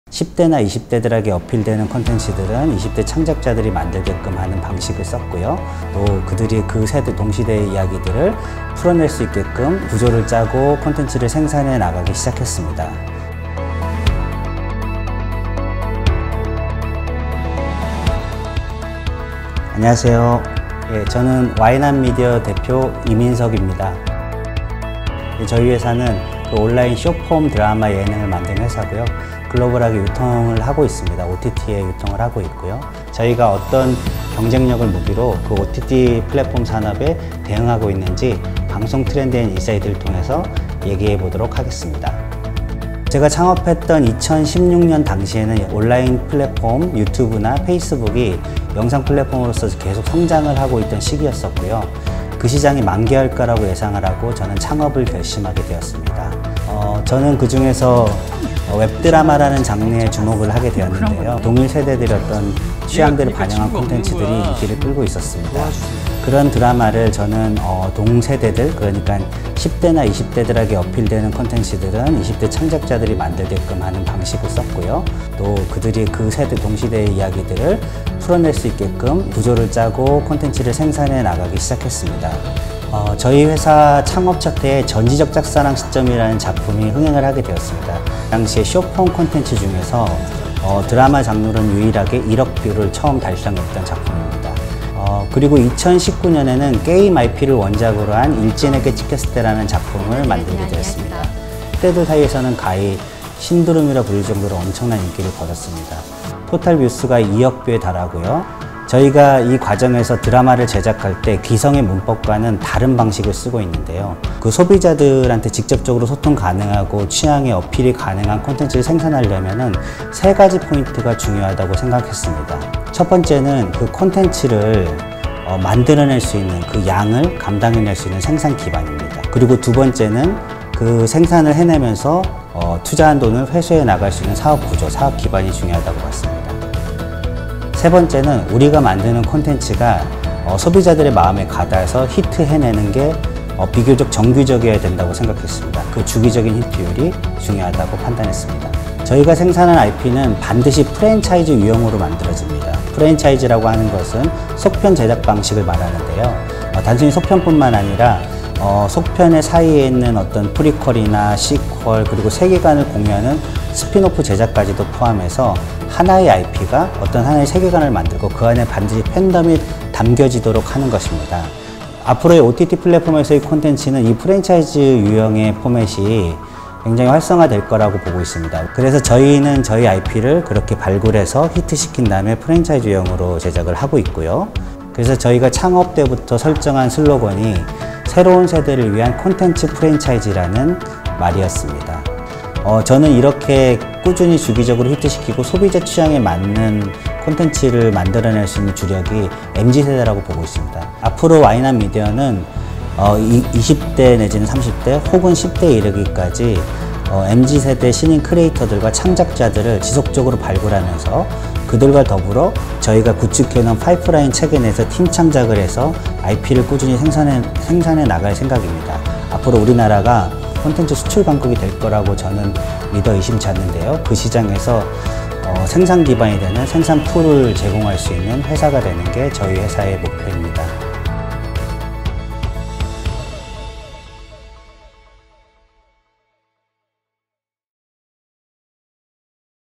[방송트렌드&인사이트 매거진 영상 인터뷰] 와이낫미디어 이민석 대표
방송트렌드&인사이트 vol.26: 콘텐츠로 헤쳐 모여!
Special Issue2 '새로운 세대를 위한 콘텐츠 프랜차이즈'
필자 와이낫 미디어 이민석 대표님을 영상으로 만나봅니다.

[전지적 짝사랑 시점], [일진에게 찍혔을 때] 등 MZ세대에게
'웹드라마 맛집' 와이낫미디어의 콘텐츠 경쟁력은 무엇일까요?

더 자세한 내용이 궁금하면?
이민석 대표님이 직접 쓴 기사를 읽어보세요!

방송트렌드&인사이트 매거진은?
콘텐츠의 힘을 믿는 방송영상산업 분야 웹 매거진입니다!